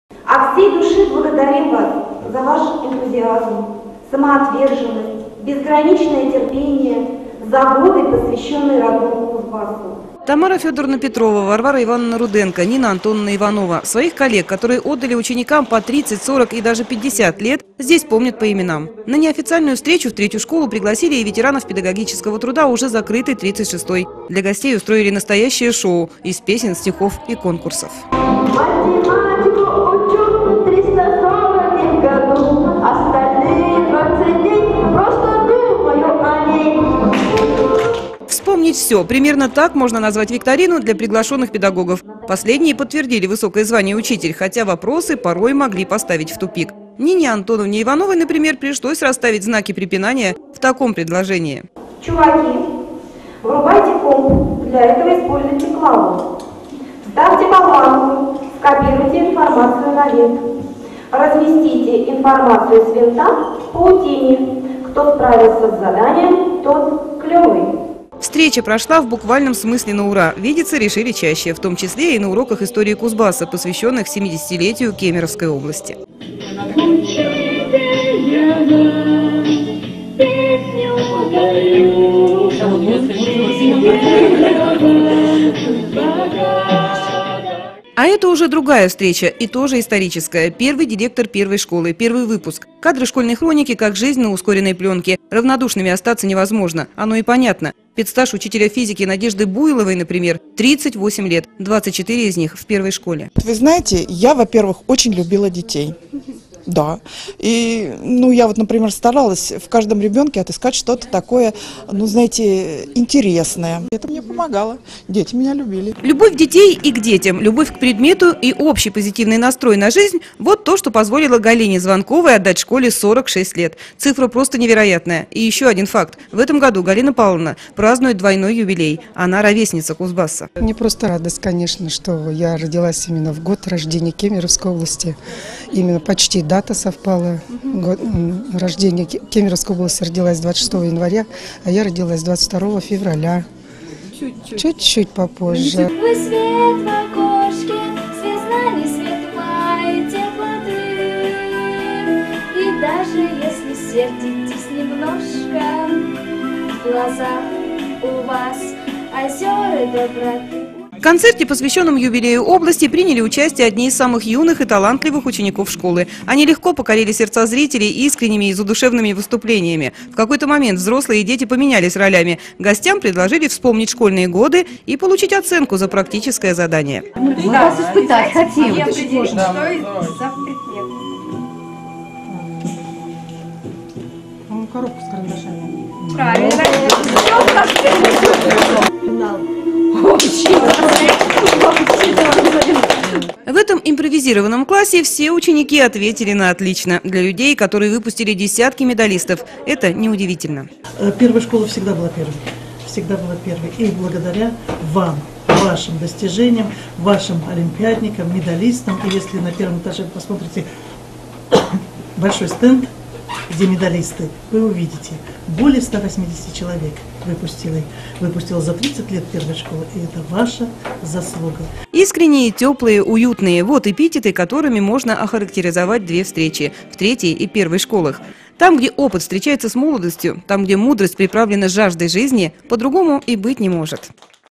Интеллектуальный потенциал Кузбасса
Время - собирать камни! В преддверие 70-летия Кемеровской области образовательные учреждения Ленинска-Кузнецкого чествуют представителей интеллектуальной элиты города. Встречи с теми, кто выучил и воспитал тысячи школьников, проходят в разных формах.